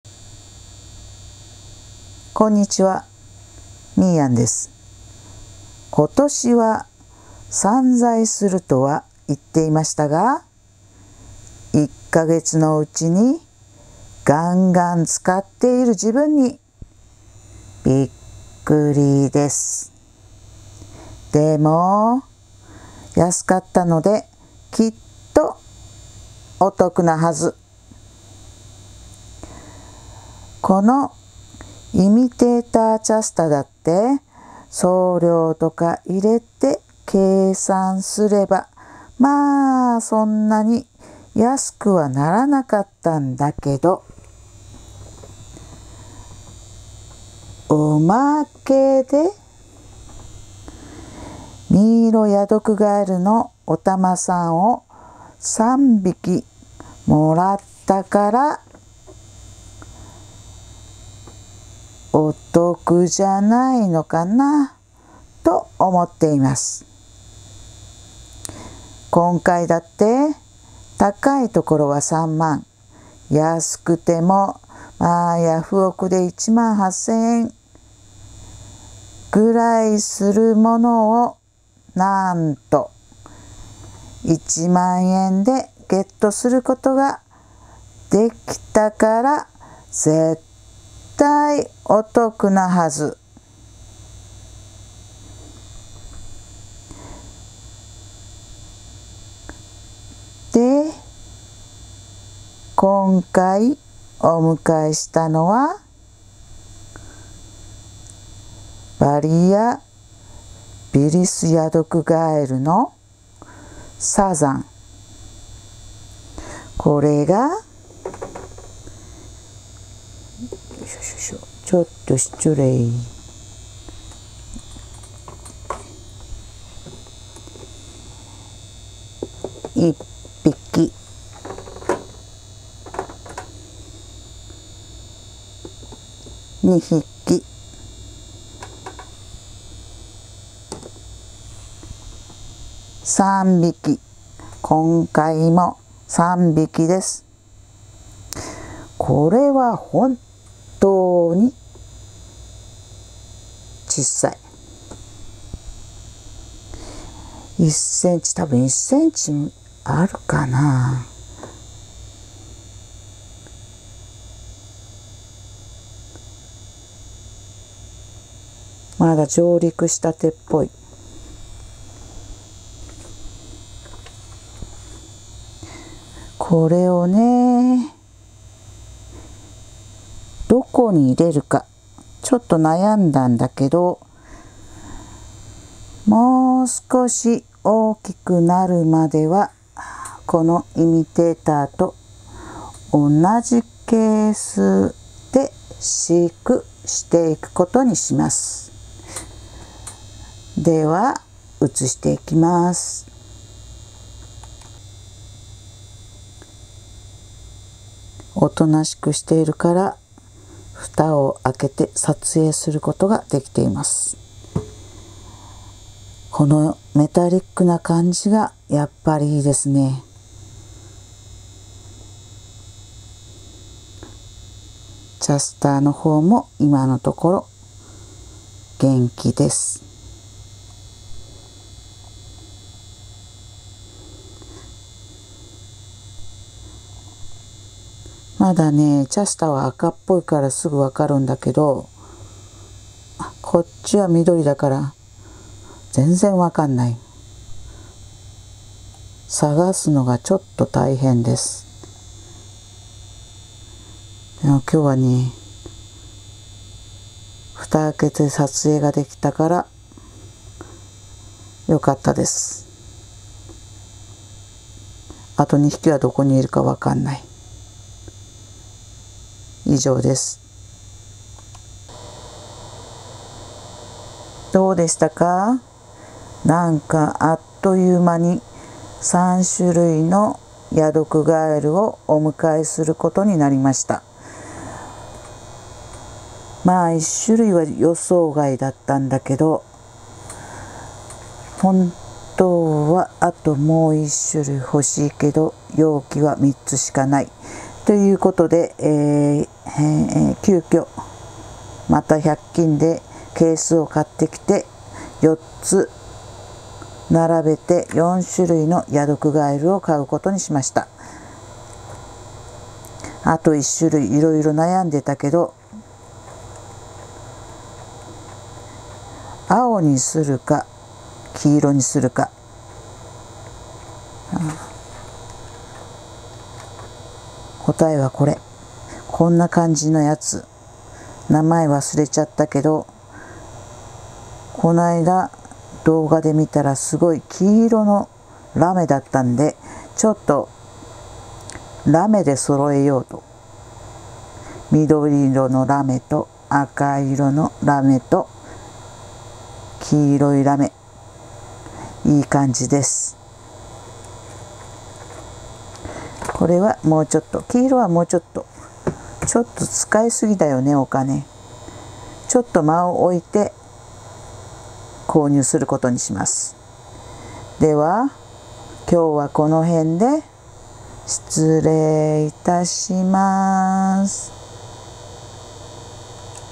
みーやん、癒しを求めて 83（ヤドクガエル編）
またまた、お迎えです。ちょっと散財しすぎなのでは？っていう話です。